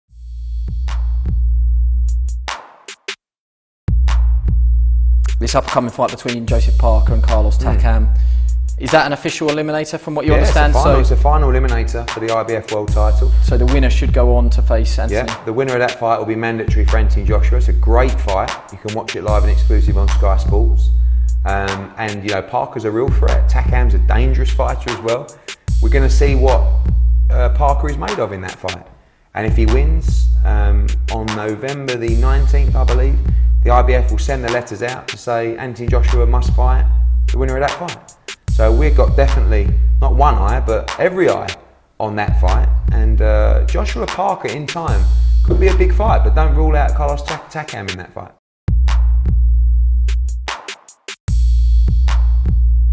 Anthony Joshua's Promoter Eddie Hearn - "Joseph Parker is a real threat"
EDDIE HEARN – ANTHONY JOSHUA’s PROMOTER:
Yeah, the winner of that fight will be the mandatory for Anthony Joshua, it’s a great fight and Parker is a real threat.  Takam is a dangerous fighter as well.  We are going to see what Parker is made of for this fight.  If he wins, on November the 19th I believe, the IBF will send letters out to say ‘Anthony Joshua must fight the winner of that fight’.  We’ve got definitely, not one eye, but every eye on that fight and Joshua/Parker in time could be a big fight but don’t rule out Carlos Takam.